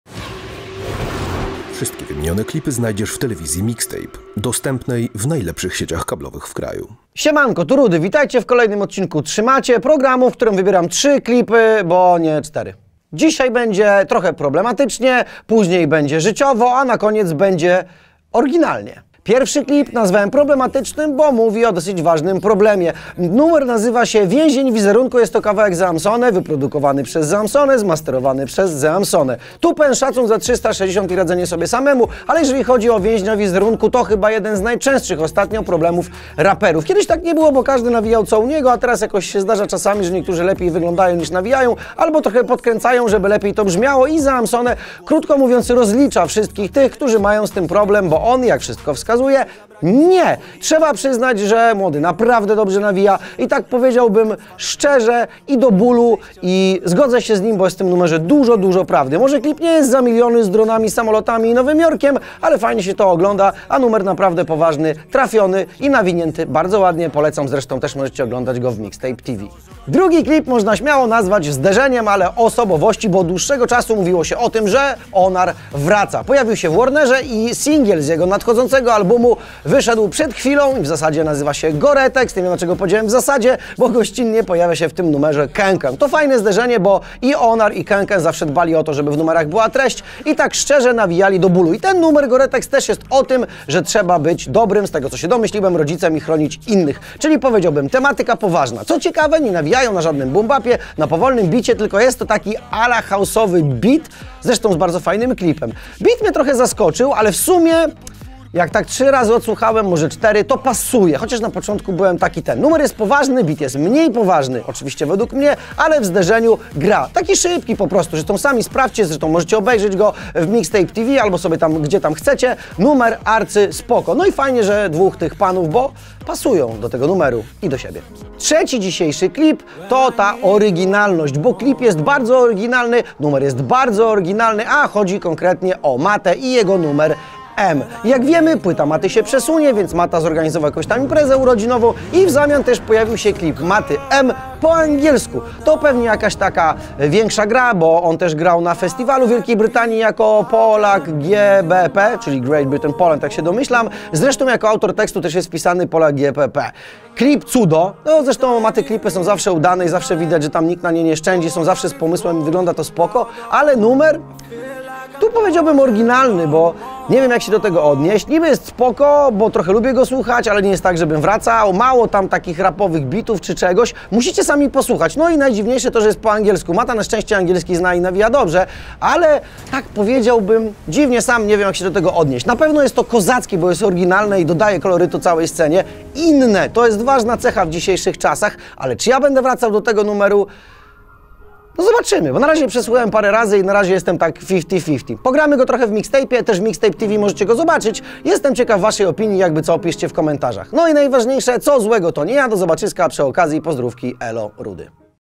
trzymacie 42 - REUPLOAD - subiektywny przegląd premier tygodnia - RAP - HIPHOP
Wracamy z cyklem trzymacie czyli subiektywnym przeglądem tygodnia, w którym prezentujemy Wam trzy, wybrane przez nas, RAPowe premiery ostatniego tygodnia. Przegląd jest subiektywny, zatem może tam nie być Twojego ulubionego klipu ;)

Oglądaj jedyny, polski kanał telewizyjny poświęcony w 100% kulturze Hip-Hop w najlepszych sieciach kablowych